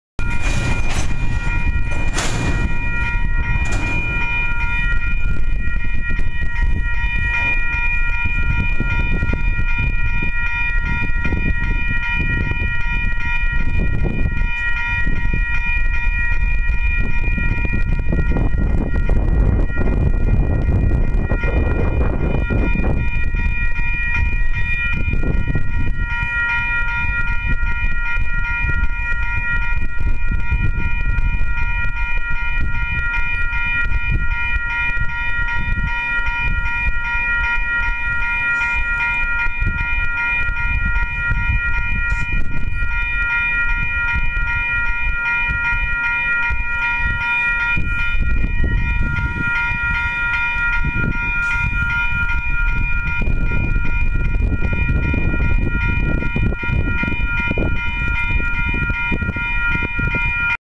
UP 8657 leads WB stack with SD70M duo, C45AH (2)
Like the next video of this 4 part series of the 8657 's movement, you could say this is an unwanted part, but it shows how I work.